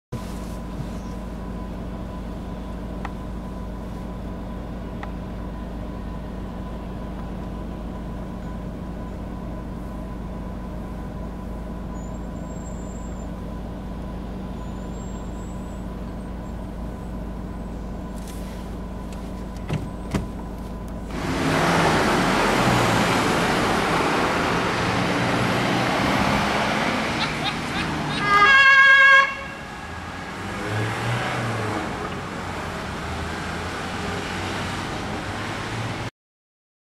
LF 16/12 + TLF 16/24-Tr + MTF FF Kerpen LZ 5
In diesem Video sieht man das Löschgruppenfahrzeug LF 16/12 , Tanklöschfahrzeug TLF 16/24-Tr , sowie das Mannschaftstransportfahrzeug MTF der Freiwilligen Feuerwehr Kerpen Löschzug 5 Sindorf, wie sie am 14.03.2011 gegen 20 Uhr auf Einsatzfahrt im Stadtgebiet sind.

Copyright : ZieglerHLF